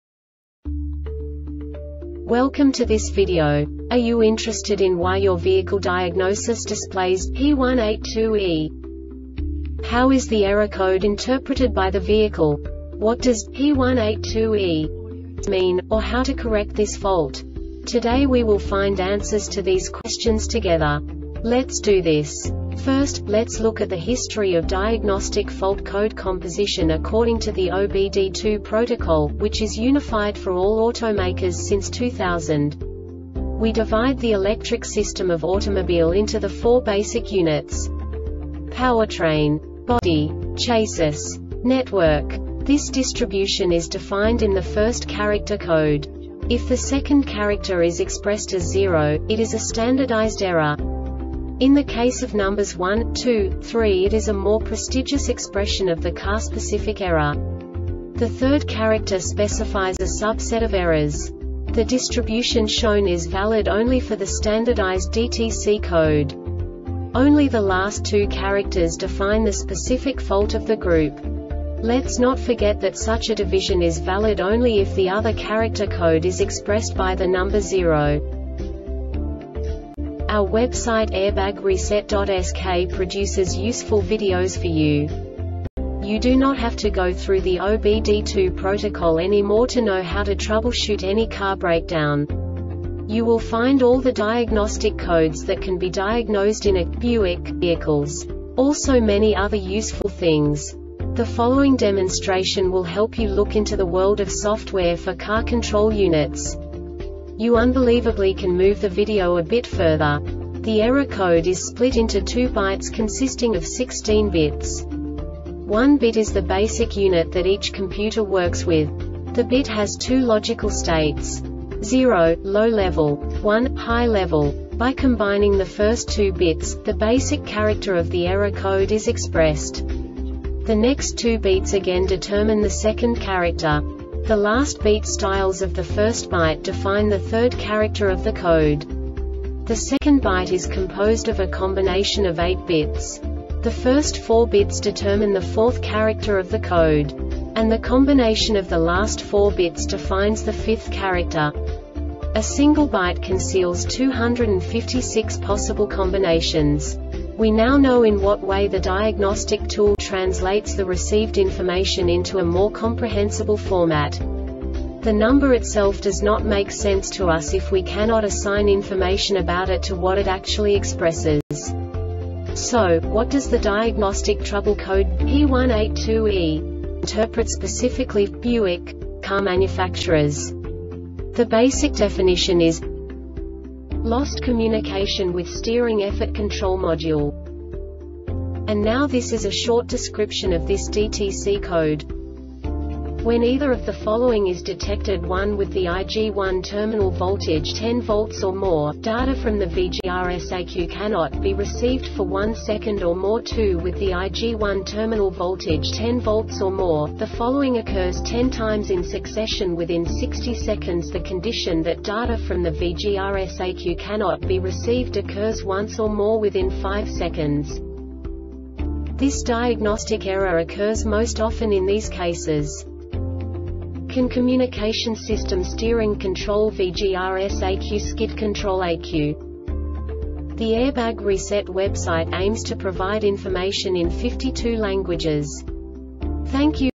DTC Buick P182E Short Explanation
Contents:
0:21 Basic DTC analysis according to OBD2 protocol standard.
1:48 Insight into programming
2:58 A diagnostically understandable description of the Diagnostic Trouble Code
3:05 Basic error definition

Make:
Buick
Code:
P182E
Definition:
Lost Communication with Steering Effort Control Module
Description:
When either of the following is detected: 1. With the IG1 terminal voltage 10 V or more,data from the VGRS ECU cannot be received for 1 second or more.2. With the IG1 terminal voltage 10 V or more,the following occurs 10 times in succession within 60 seconds. The condition that data from the VGRS ECU cannot be received occurs once or more within 5 seconds.
Cause:
 CAN communication system Steering control (VGRS) ECU Skid control ECU